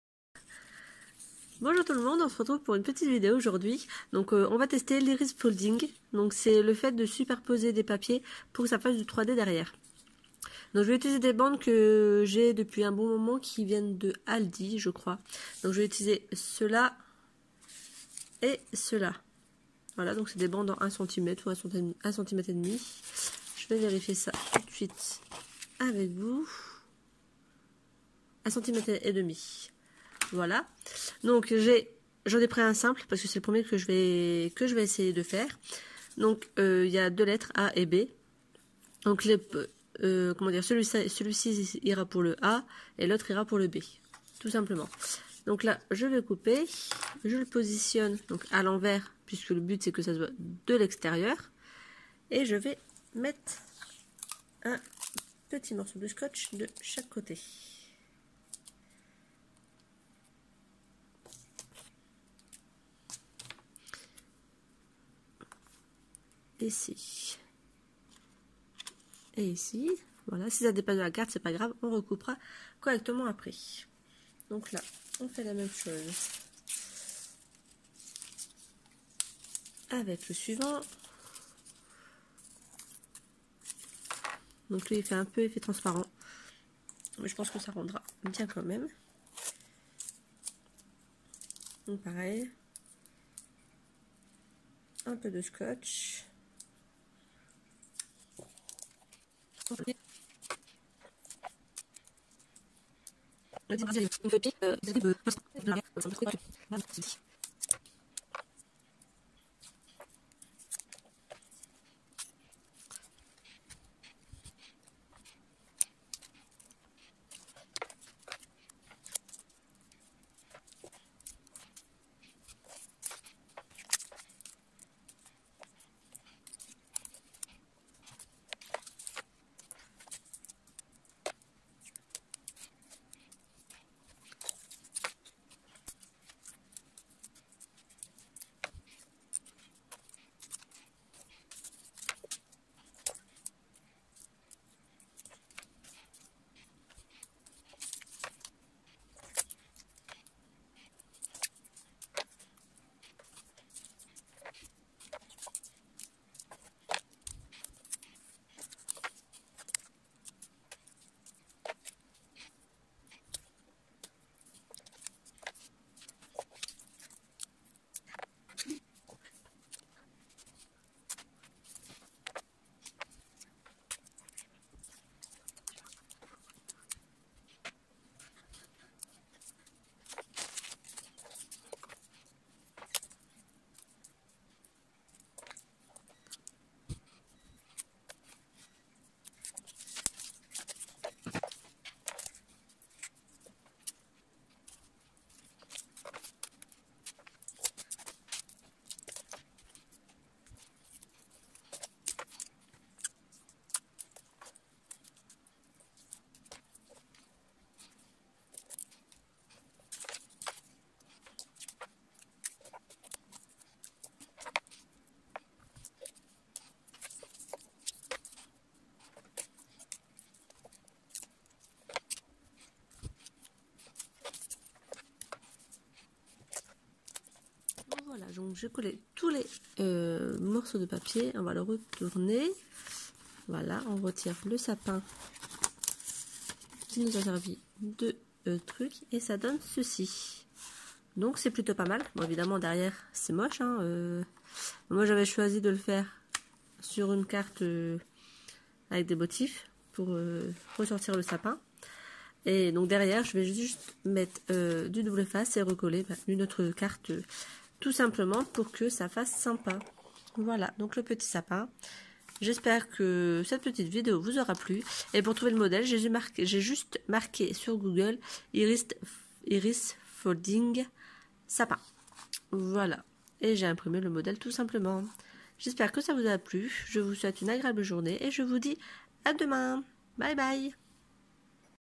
carte sapin iris folding *en route pour Noël J16*
coucou premier test d'iris folding, j'espère que ça vous plaira :) laissez moi un petit commentaire :)

Bonne journée